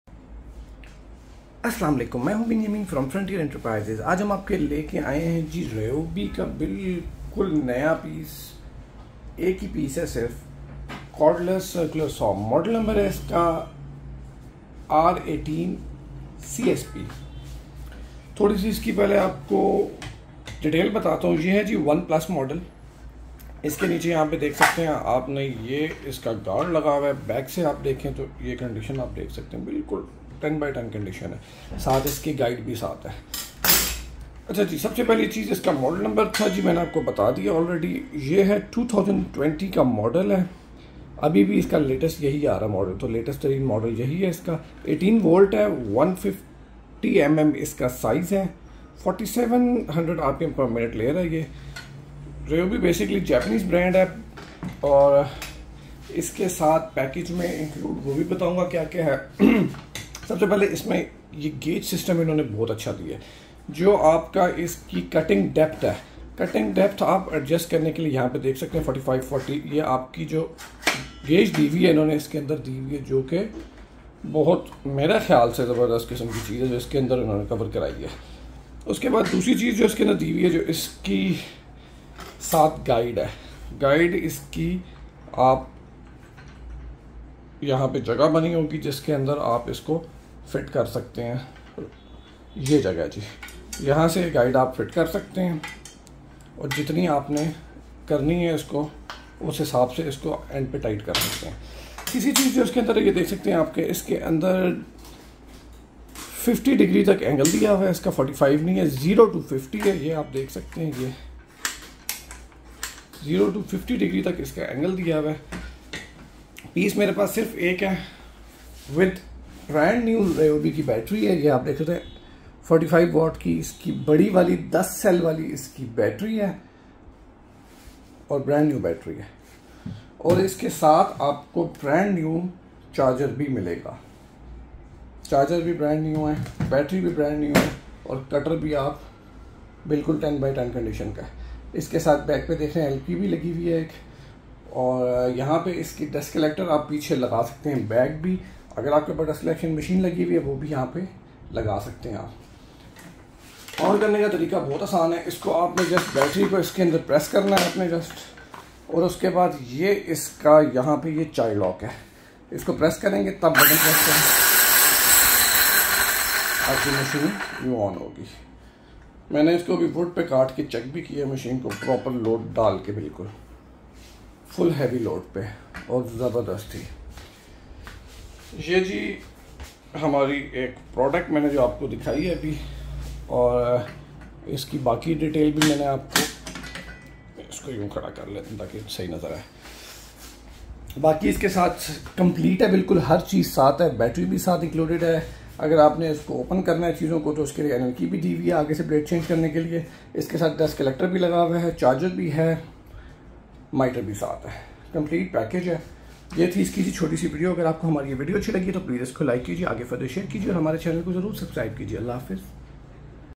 RYOBI R18CSP 18V 150MM CORDLESS CIRCULAR SAW Review and stock by FE
RYOBI R18CSP 18V 150MM CORDLESS CIRCULAR SAW
18v
150mm
includes
1x cordless circular saw
1x guide
1x brand new 48w battery
Frontier Enterprises
212 adamjee road saddar rwp